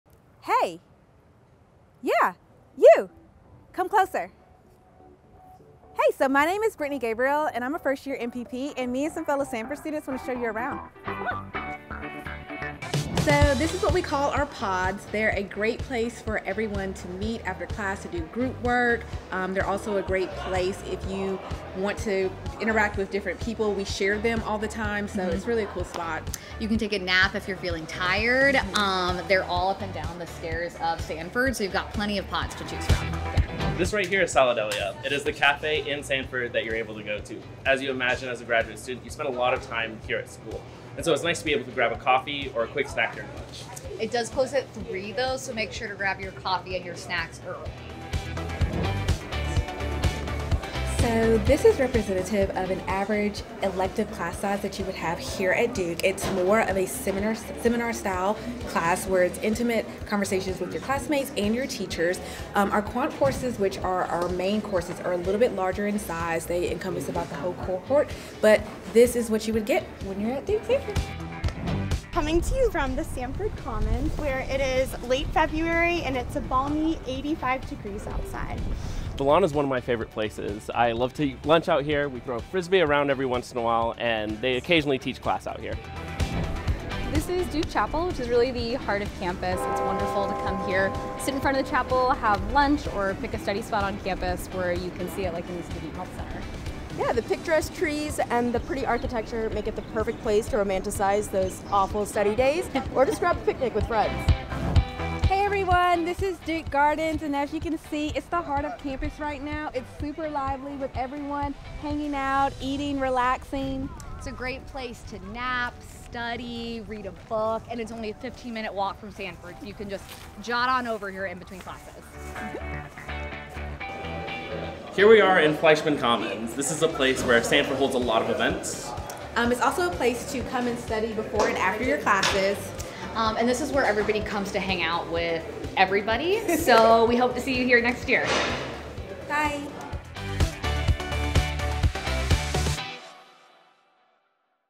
Duke Sanford Master of Public Policy Virtual Tour
Follow along with these MPP students as they take you around the Sanford School of Public Policy and Duke's campus!